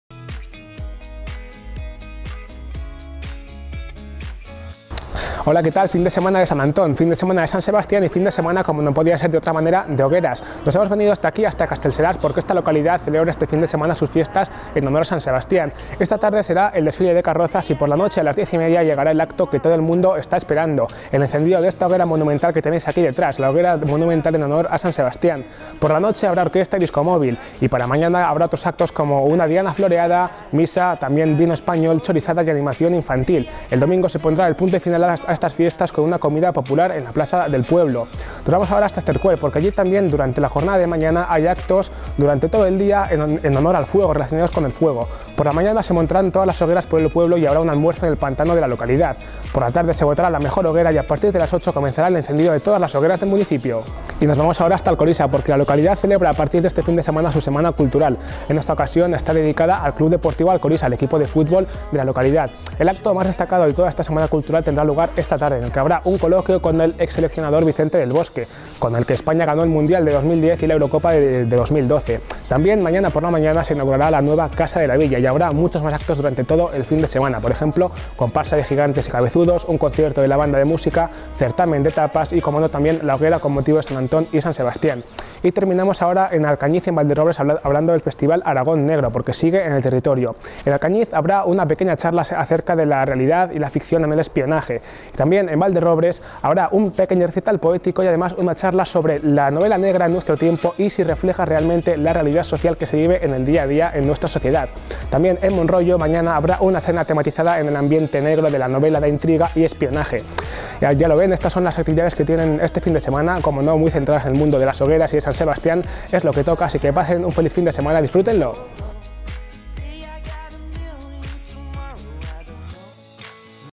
Agenda cultural fin de semana 20 y 21 enero 2018
Agenda cultural para el fin de semana del 20 y 21 de enero de 2018 donde destacan las hogueras en honor a San Antón y San Sebastián. Hoy Adrián Monserrate se encuentra en Castelserás donde se encenderá la monumental hoguera en honor al Patrón San Sebastián.